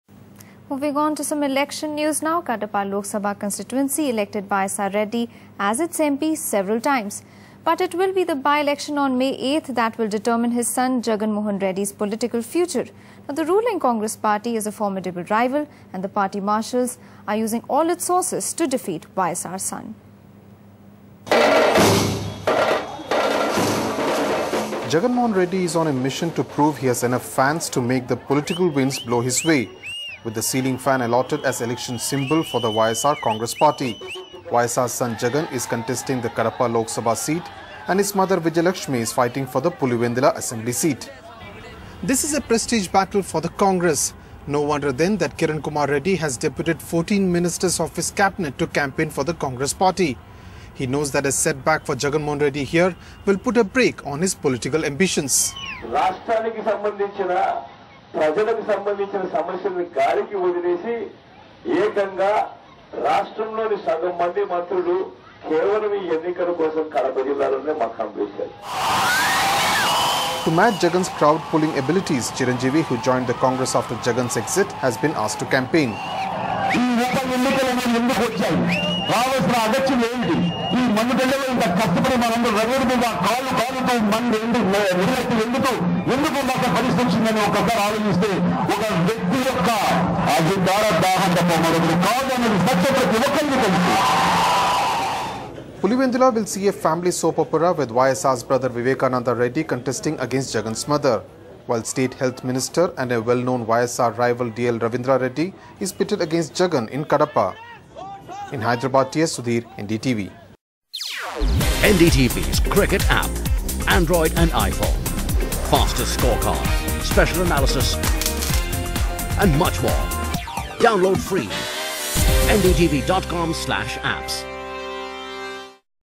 Jagan, Congress gear up for poll battle
Kadapa Lok Sabha constituency elected YSR Reddy as its MP several times but it will be the by-election on May 8 that will determine his son Jaganmohan Reddy's political future. The ruling Congress party, a formidable rival, marshals all its resources to defeat YSR's son.